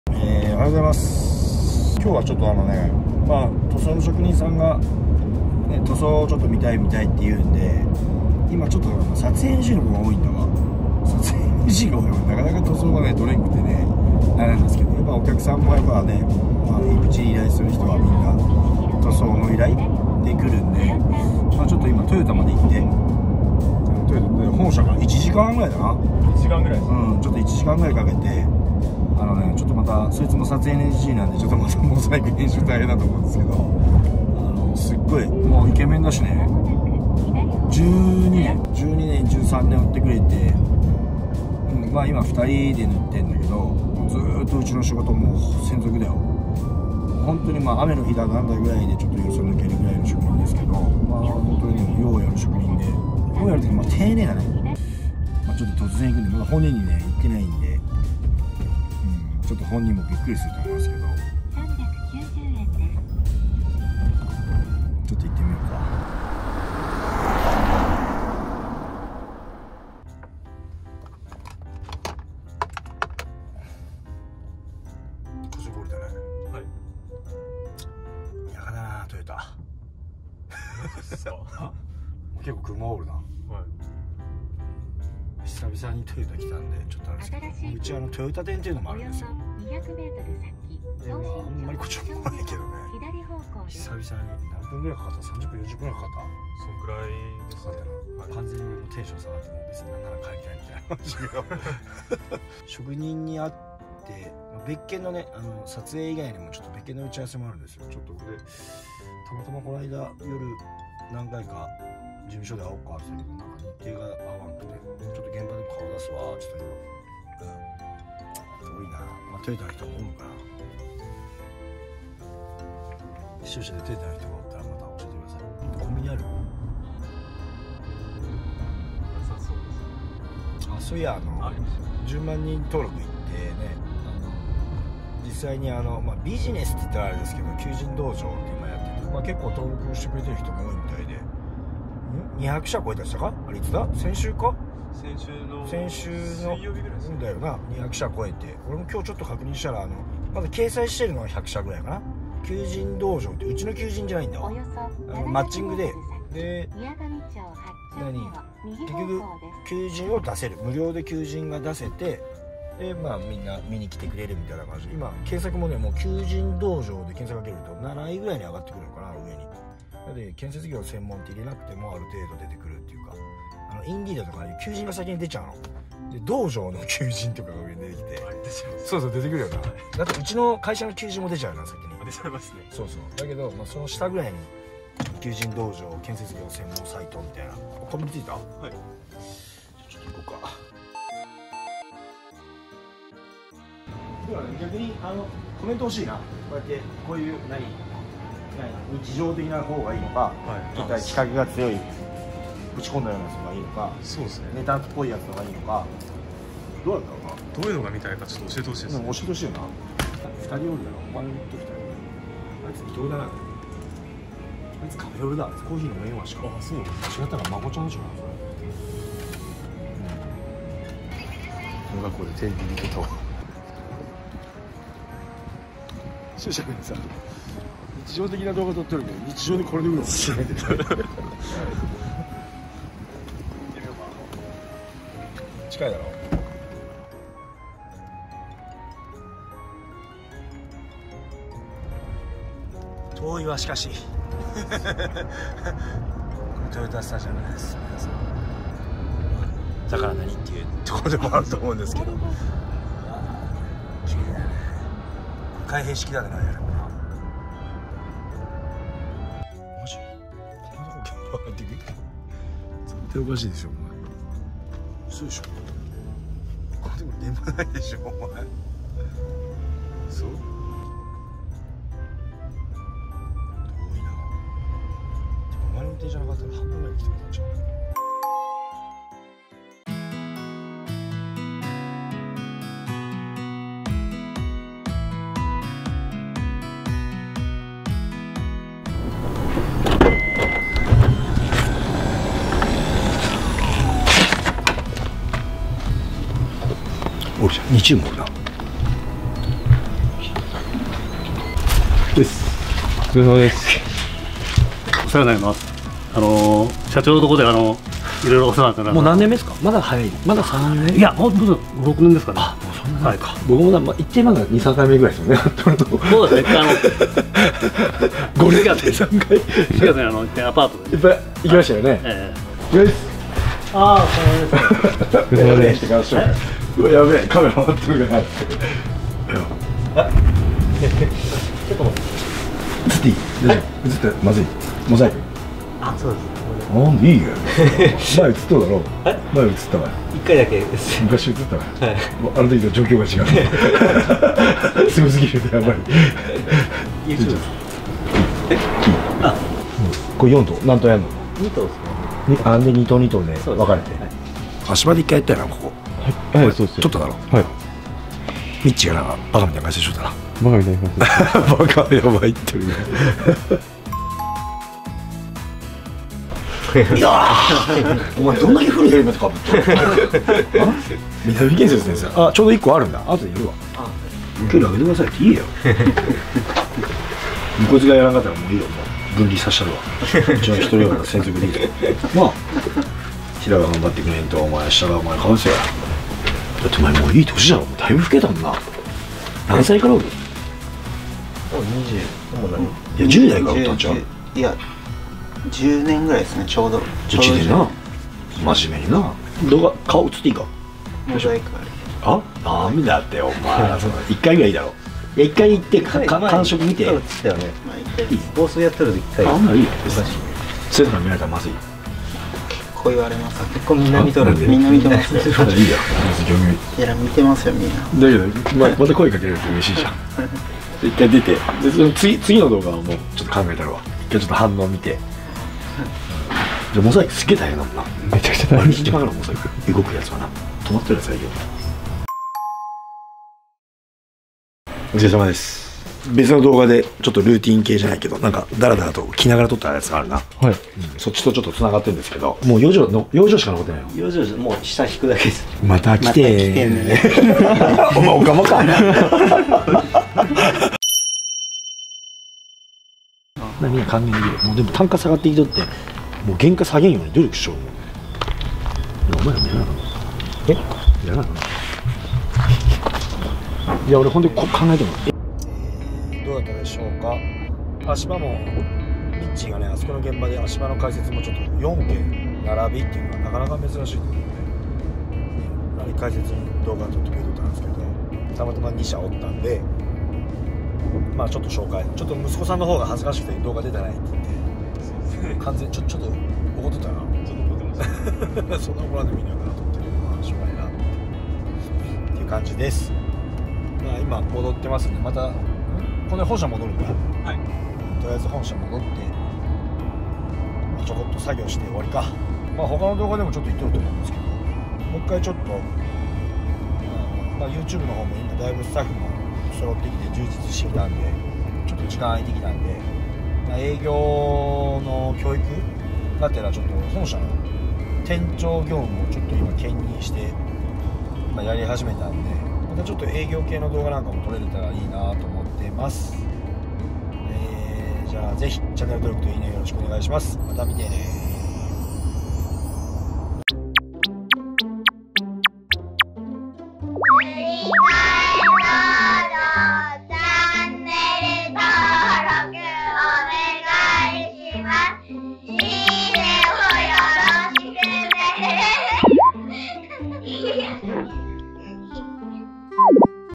建設業現場系youtuber、撮影前のルーティン | 撮影開始から撮影後まで。
今回は、現場撮影前に行っているルーティン動画を撮ってみました。
好評、要望がありましたら、職人編や経営者編も撮りたいと思います！